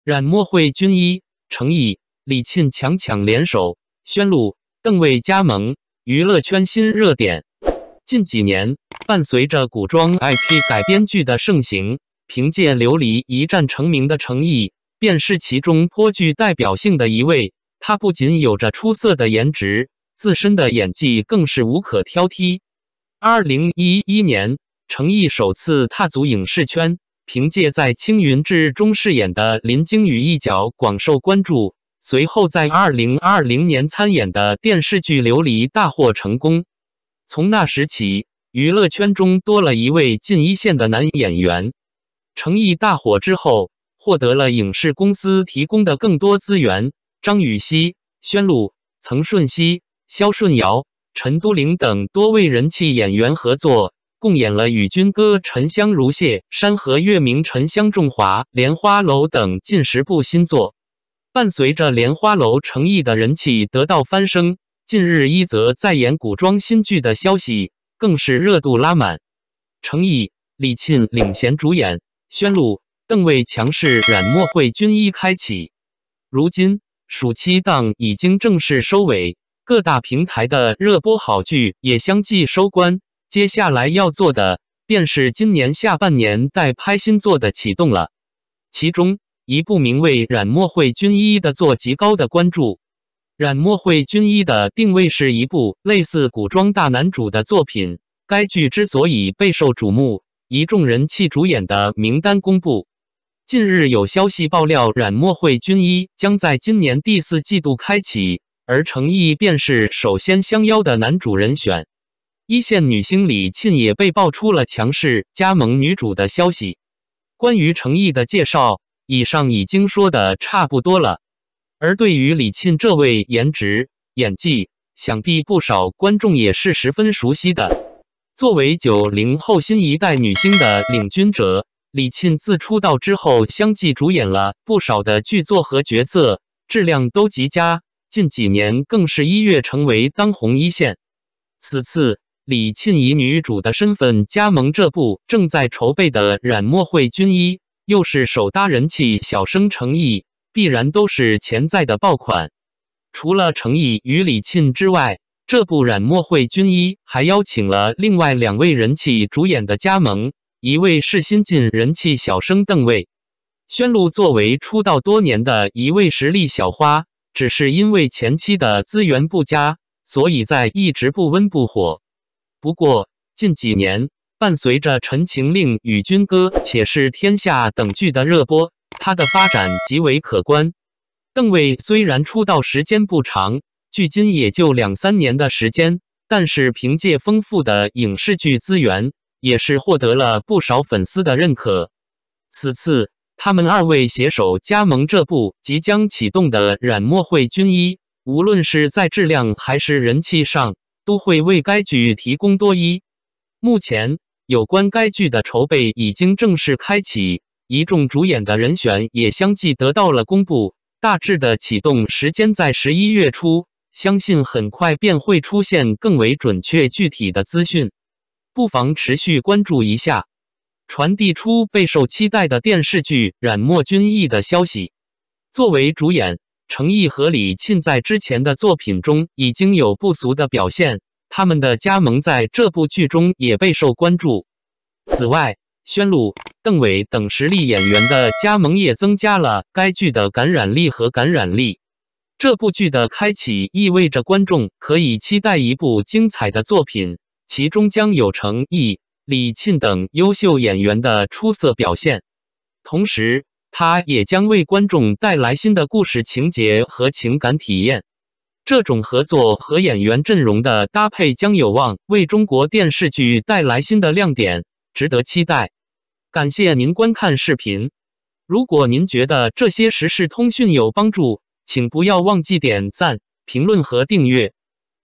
《染墨绘君衣》：成毅、李沁 强强联手，宣璐、邓为加盟，娱乐圈新热点！chengyi
成毅、李沁领衔主演，宣璐、邓为强势加盟，《染墨绘君衣》开启